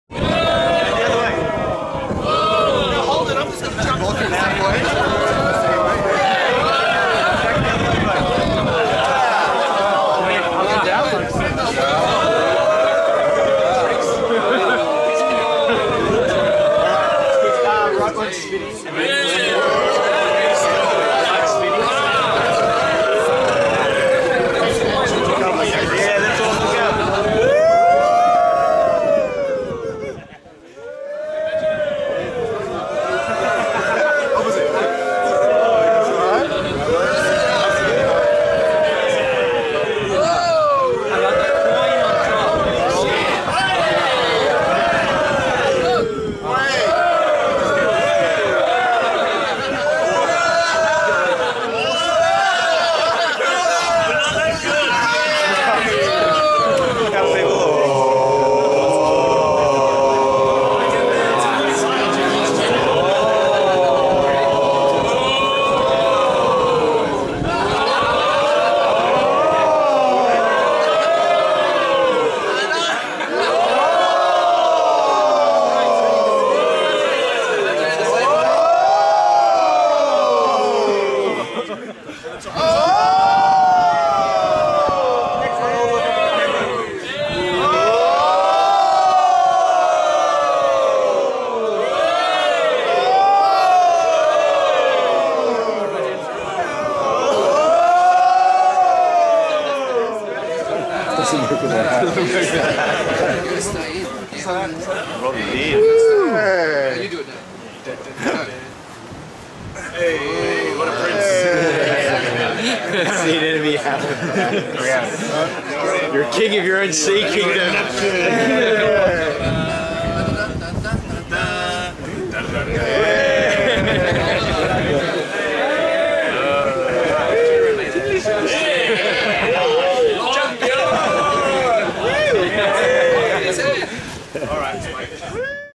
Thumb circles of 6
6 men stand in a circle. All the left hands hold the thumb of the man next to them. Likewise with the right. They play. They improvise. They have fun. It is a technique used for crowd warming, ritual preparation, and for fun.